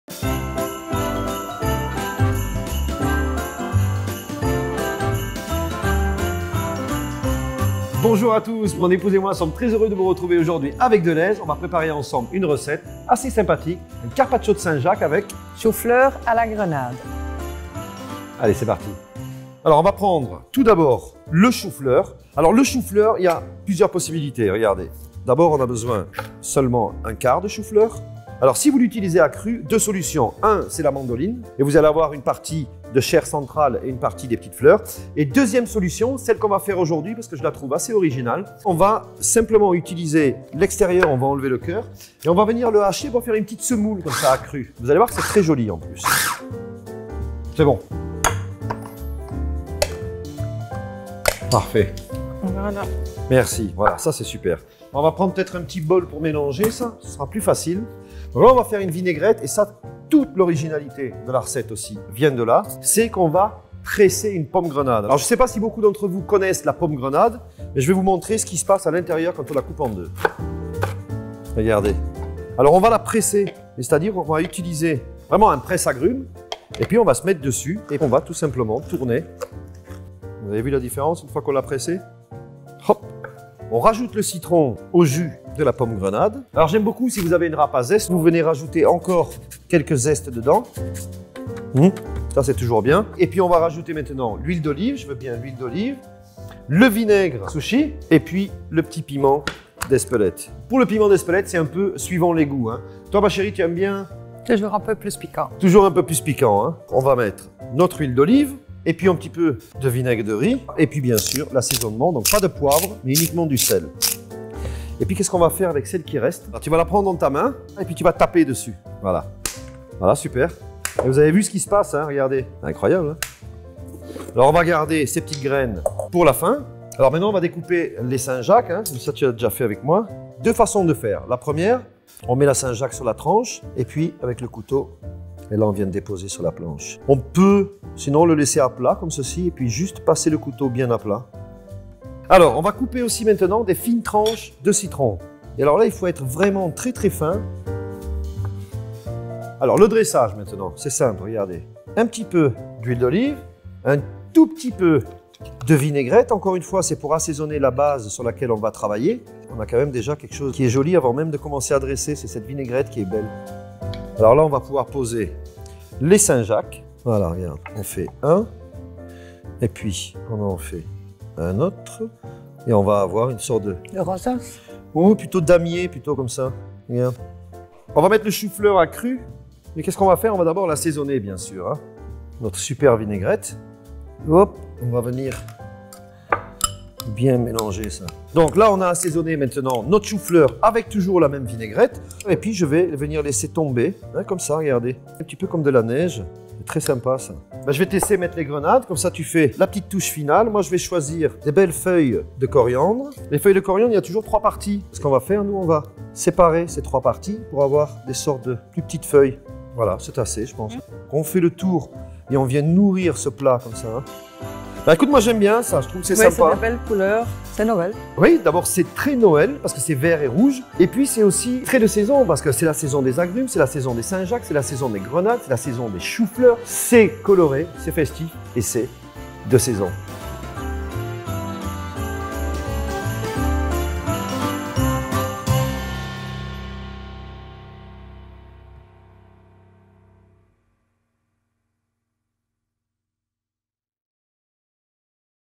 Carpaccio de noix de Saint-Jacques et de chou-fleur à la grenade par David Martin
Envie de régaler vos invités avec une délicieuse entrée ? Alors, ce carpaccio frais est fait pour vous. Cette recette vous vaudra sûrement beaucoup de 'Ohhh' et de 'Ahhhh'. Bon appétit !

Ingrédients pour 4 portions:

200 g de noix de Saint‑Jacques
1/4 de chou‑fleur
1 citron vert
1/2 grenade
1/4 botte de coriandre
1/2 citron vert (pour la vinaigrette)
1/2 grenade (pour la vinaigrette)
4 c. à s. d’huile d’olive (pour la vinaigrette)
piment d'Espelette (pour la vinaigrette)
sel (pour la vinaigrette)

Préparez la vinaigrette : pressez le jus de la demi‑grenade dans un presse‑agrumes. Prélevez‑en 2 c.à.s., ajoutez‑lui le jus du citron vert et le reste des ingrédients indiqués.
Taillez le chou‑fleur en fines tranches, à la mandoline. Coupez les noix de Saint‑Jacques et le citron vert en très fines tranches. Disposez le tout sur un grand plat ou sur des assiettes individuelles. Parsemez de grains de grenade et de feuilles de coriandre.
Arrosez de vinaigrette et dégustez aussitôt. Vous pouvez aussi la présenter à part et laisser chacun en ajouter selon ses goûts.

Astuce : Pour faciliter la découpe des noix de Saint-Jacques, vous pouvez les mettre 1 h. avant au congélateur pour les raffermir.